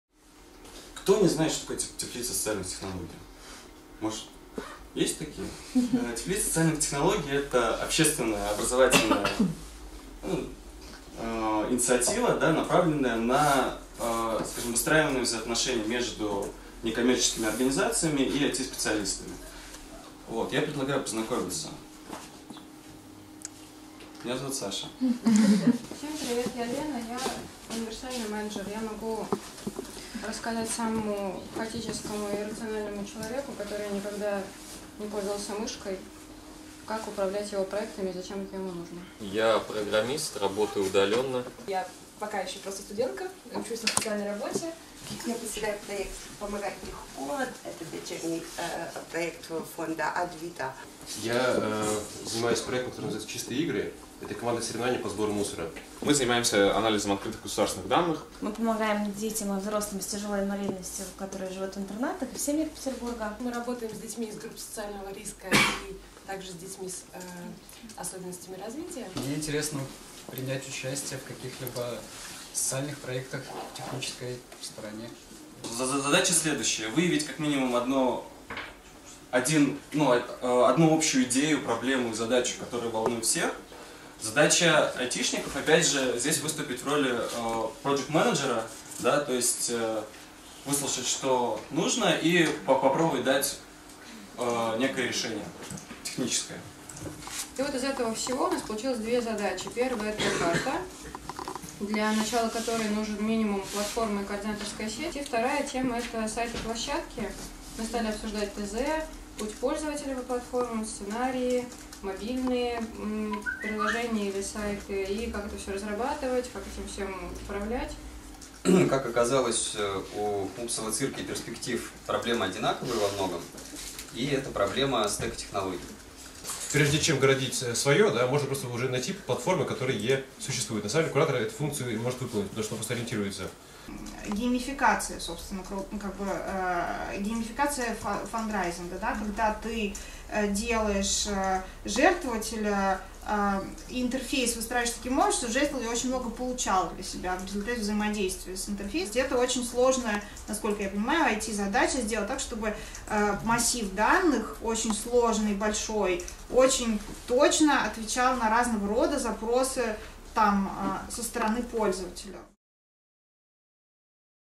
Митап Теплицы в Санкт-Петербурге.
3 апреля 2016 в Петербурге при поддержке общественного коворкинга «Открытое пространство»  прошёл митап Теплицы «Общественные организации и IT-сообщество: идеи и варианты сотрудничества».